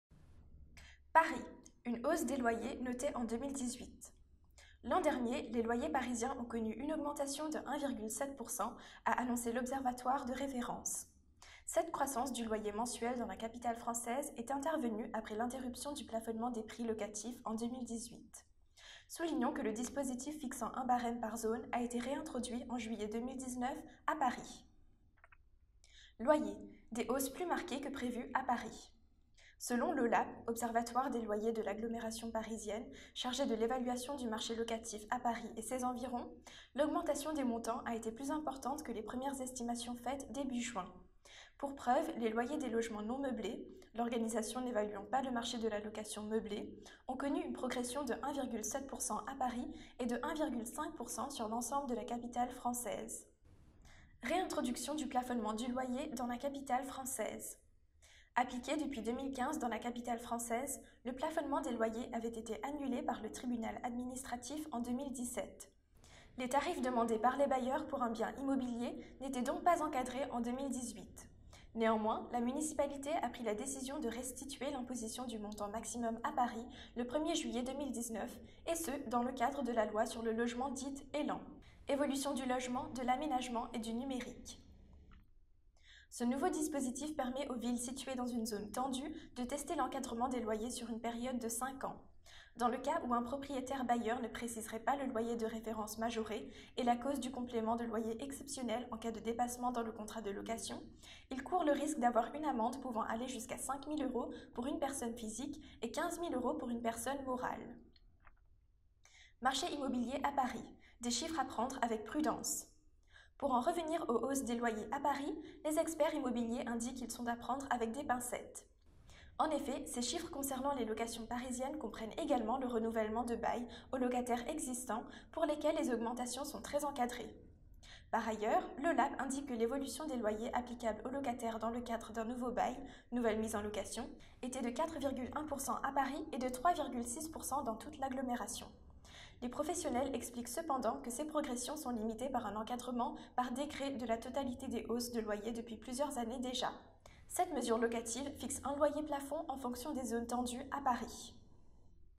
Paris : les loyers des logements non meublés ont augmenté en 2018
L’OLAP a noté une hausse des loyers des logements non meublés à Paris et dans l’ensemble de la capitale française, en 2018 Cependant, la municipalité a décidé de réintroduire le plafonnement des loyers à Paris le 1er juillet 2019, et ce, en accord avec la loi dite Elan (Évolution du logement, de l’aménagement et du numérique).